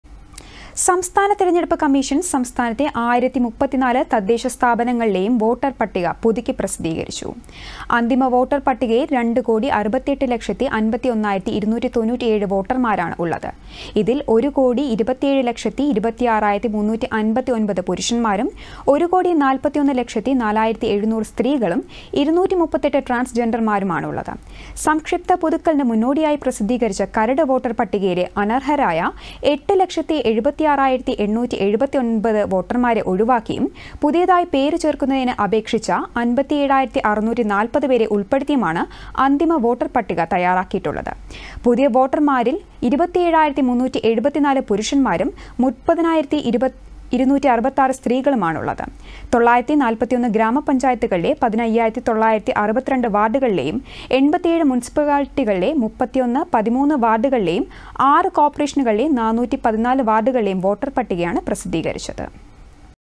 സംസ്ഥാനത്തെ തദ്ദേശസ്ഥാപനങ്ങളിലെ വോട്ടർപട്ടിക പുതുക്കി പ്രസിദ്ധീകരിച്ചു
അന്തിമ വോട്ടർപട്ടികയിൽ, 2 കോടി 68 ലക്ഷത്തി 51,297 വോട്ടർമാരാണുള്ളത്.